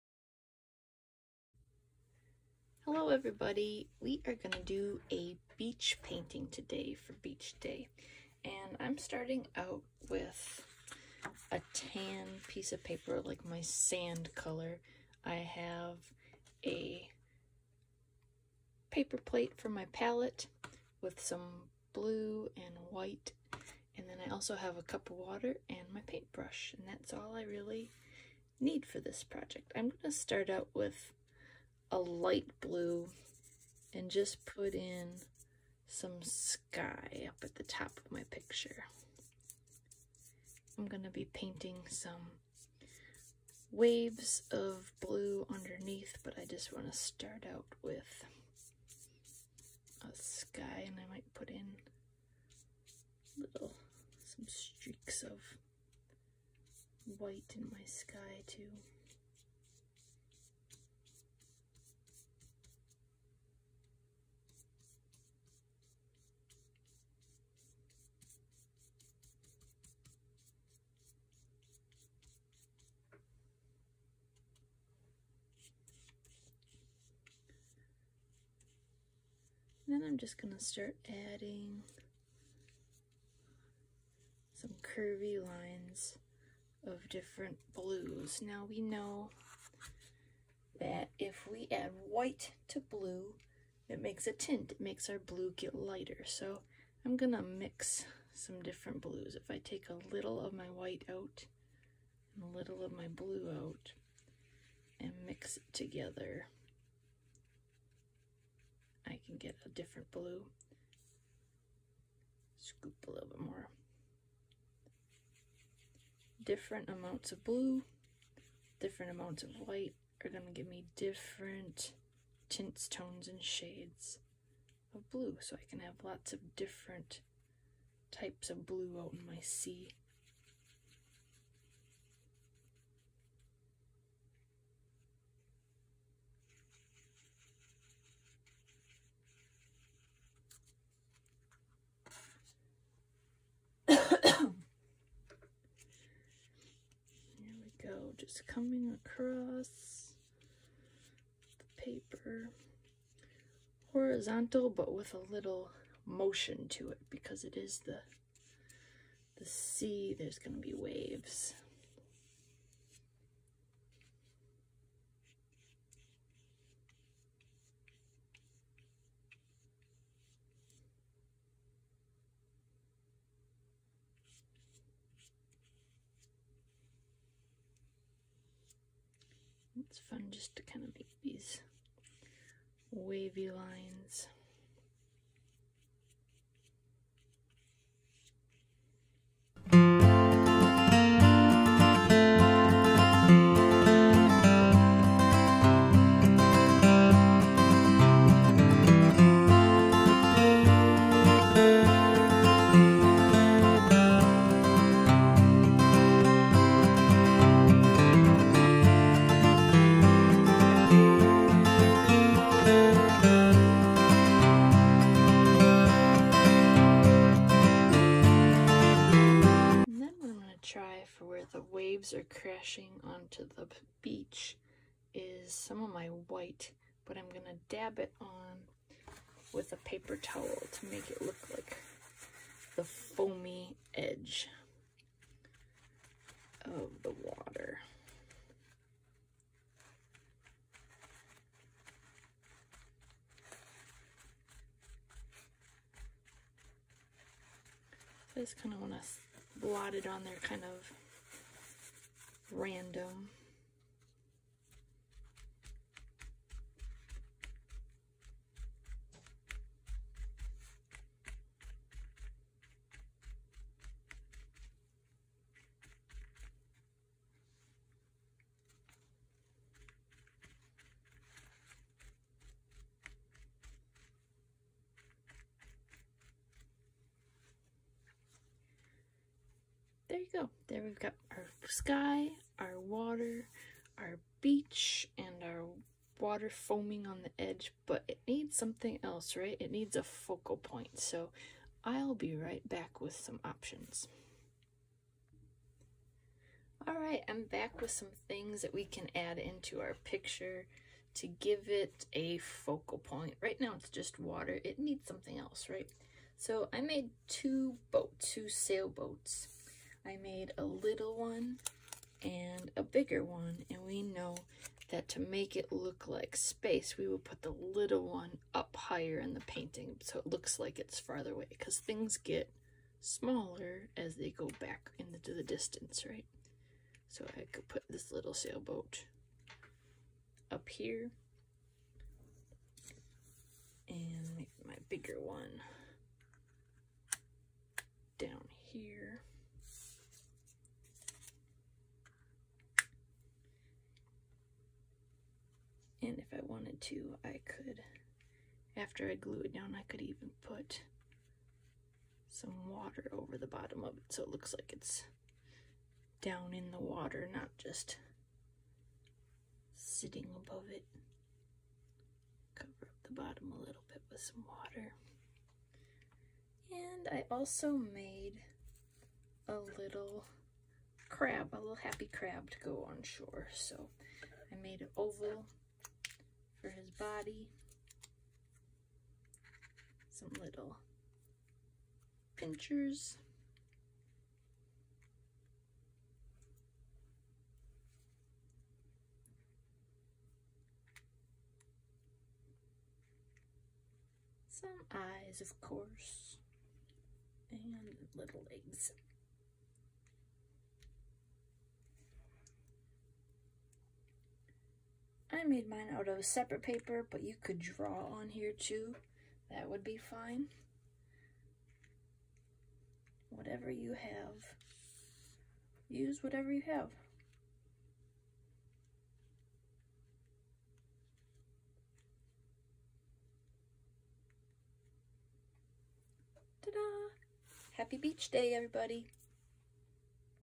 Beach Day Painting
Paint along with me to create a beach scene. If we can’t be at the beach, let’s just paint one!
Materials I used:
Tan paper
Blue and white acrylic or tempera paint
Paint brush
Paper Towel
Scraps of colored paper
Scissors
Glue
However, you can use what you have. You could create something similar with watercolors or a collage all with paper.